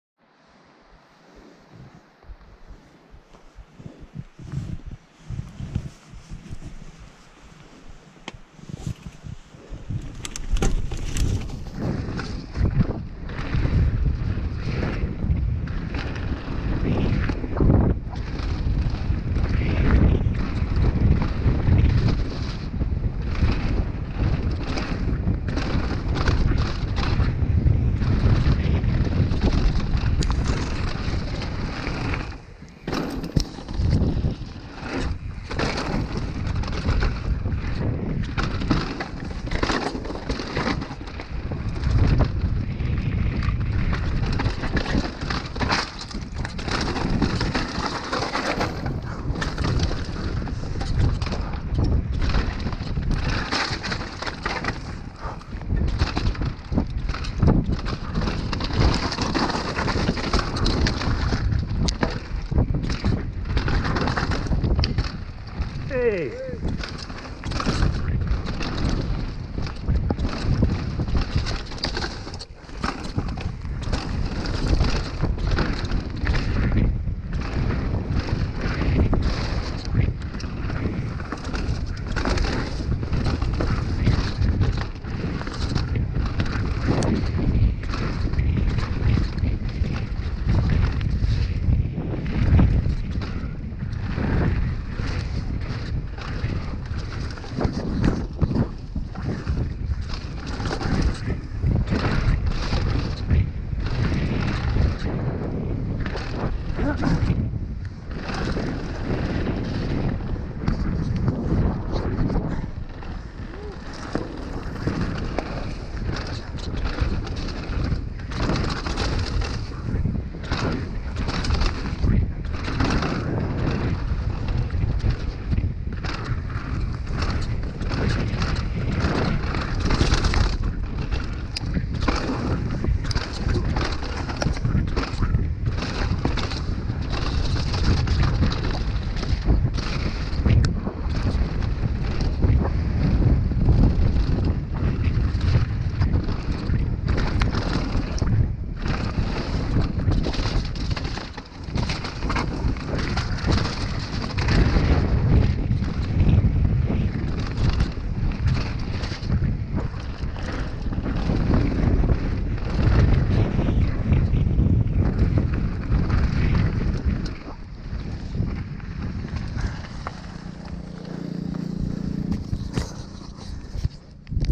Revolution Bike Park. Ffar Side DH. Full Run
Revolution Bike Park/ Llangynog. Ffar Side DH - Full Run.

Take a lap with me down the Ffar Side DH track at Revolution Bike Park. Video from mid Feb, sorry for the slightly blurry footage, as you can see it was a pretty miserable day.